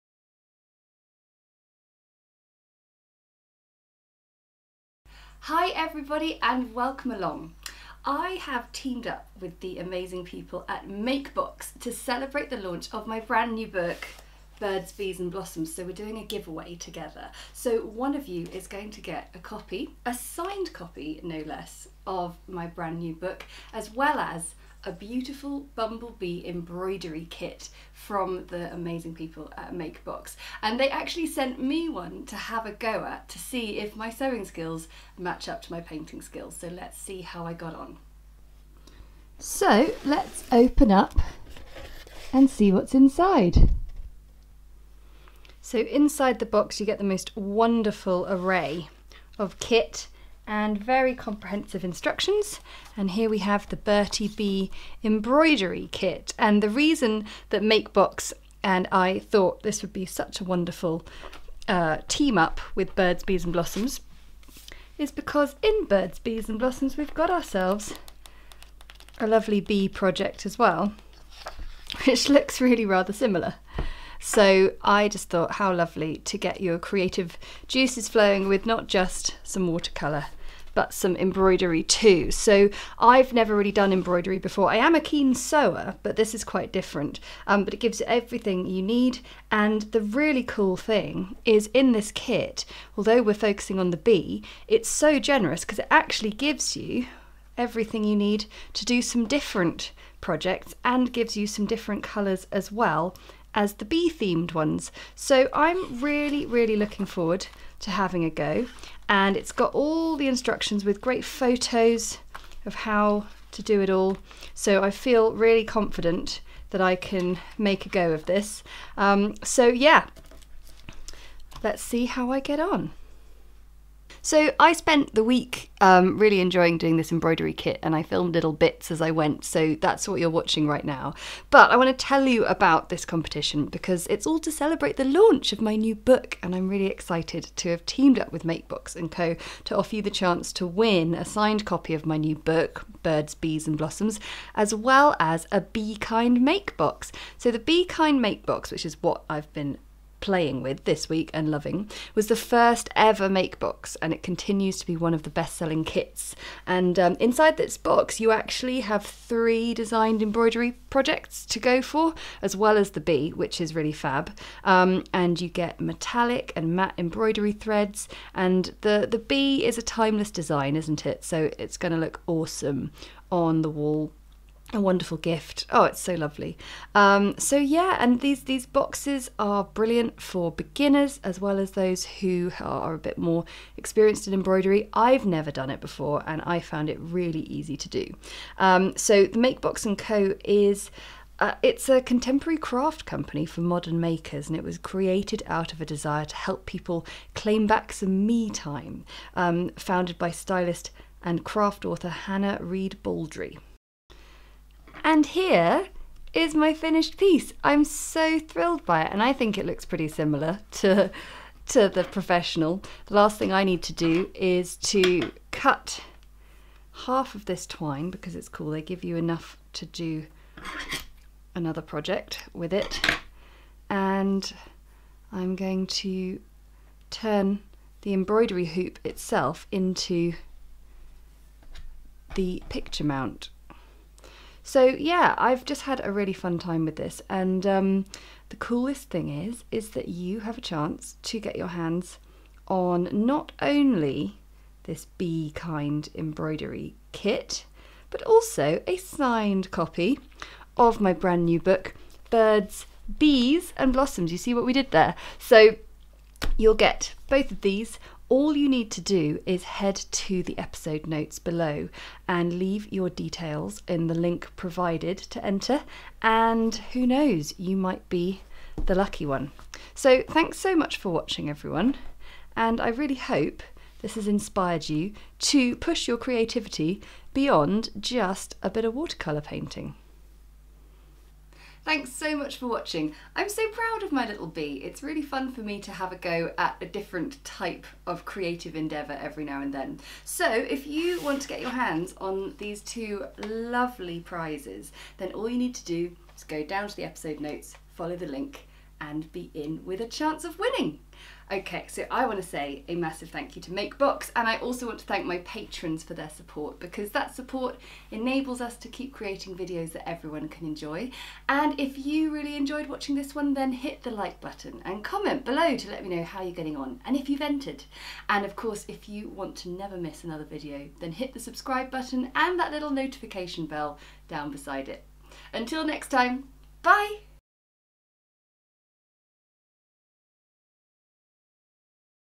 BIRDS, BEES & BLOSSOMS GIVEAWAY
UPDATE: ***We have a WINNER! Our winner has been contacted directly by MakeBox to claim their prize! Thank you everyone for entering! If you didn't win this time, never mind, keep your eyes peeled for more giveaways in future!***

All of our favourite things! !🐝🕊 I've teamed up with the wonderful team at Makebox & co to bring you a stunning birds and bees giveaway worth £45:

What you will win:

MakeBox & Co
The Bee Kind Embroidery
Posey Embroidery
Bee Kind Postcard Embroidery
Including: 7 inch embroidery hoop/ 6 inch wooden embroidery hoop/ Grey natural linen square/ Ink blue natural linen square/ Threads in pink, pale pink, green, black, silver and gold/ Embroidery needle and pins/ A4 sheet of carbon paper/ Felt square backing/ Bakers twine, to hang

Harriet De Winton
Birds Bees and Blossoms step-by-step watercolour book signed copy

One lucky winner will be selected by the 24th of October. Remember we will contact you directly and to be aware about scam accounts.